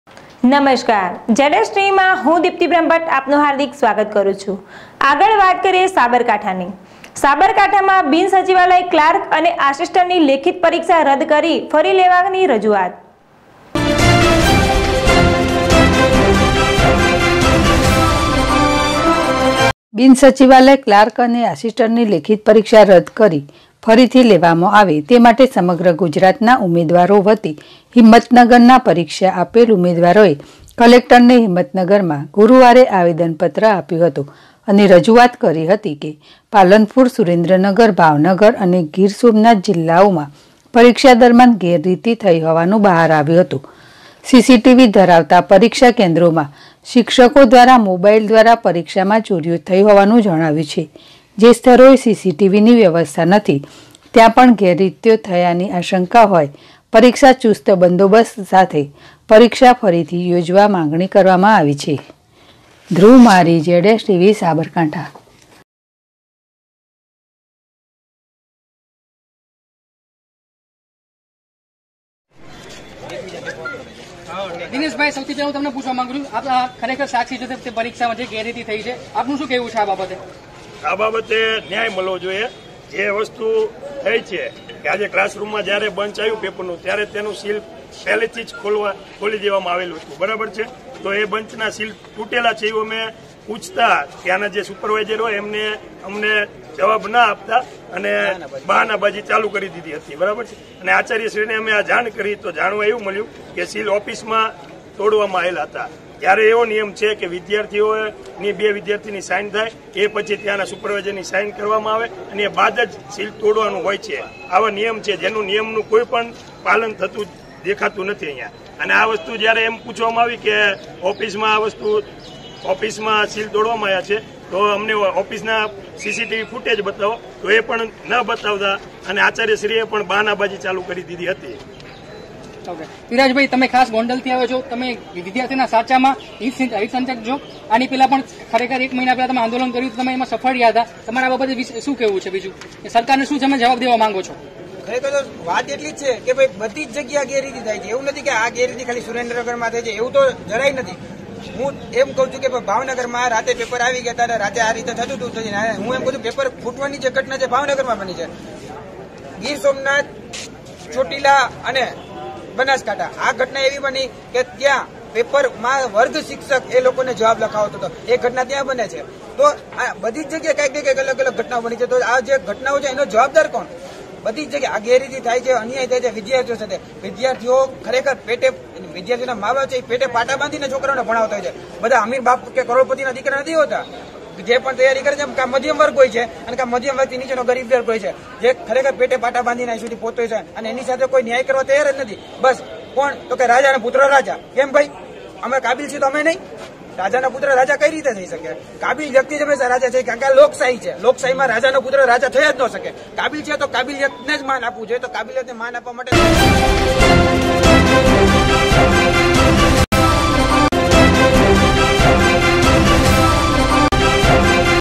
સાબરકાંઠા:- બિન સચિવાલય ક્લાર્ક અને આસીસ્ટન્ટની લેખિત પરીક્ષા રદ કરી
સાબરકાંઠા:- બિન સચિવાલય ક્લાર્ક અને આસીસ્ટન્ટની લેખિત પરીક્ષા રદ કરી ફરીથી લેવામાં આવે તે માટે સમગ્ર ગુજરાતના ઉમેદવારો વતી હિંમતનગરના પરીક્ષા આપેલ ઉમેદવારોએ કલેક્ટરને હિંમતનગરમાં ગુરુવારે આવેદનપત્ર આપ્યું //ZSTVNEWS//